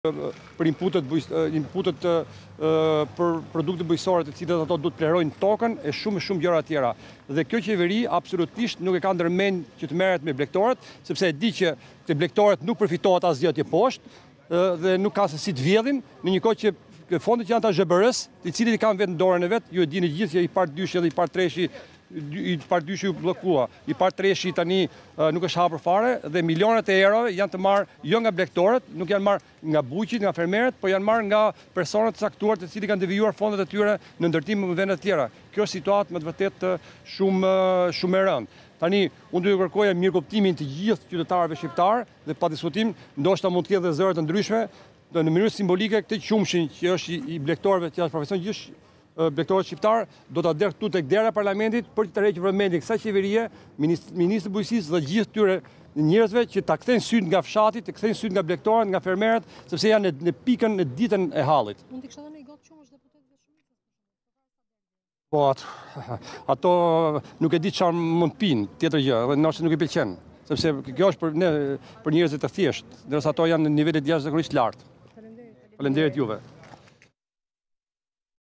“Milionat e eurove nuk janë marrë nga blegtorët, por...”-Sula: Fondet e AZHBR-së i kanë vetë në dorë
Deputeti i Partisë Demokratike, Dashnor Sula, i cili është solidarizuar me protestën e blegtorëve tha në një prononcim për mediat se fondet që jepen për fermerët i marrin persona të caktuar.

Në një prononcim për mediat para nisjes së seancën parlamentare Sula tha se situata me fermerët është e rendë, duke i bërë thirrje ministrisë së Bujqësisë që të kthejë vëmendjen nga fshati.

“Kjo qeveri nuk e ka ndërmend të merret me blegtoret. Fondet janë të AZHBR dhe i kanë vetë në dorë. Ju e dini që IPARD 2 u bllokua, IPARD 3 nuk është hapur fare dhe milionat e eurove nuk janë marrë nga blegtorët e fermerët, por nga persona të caktuar që i kanë devijuar fondet në ndërtim.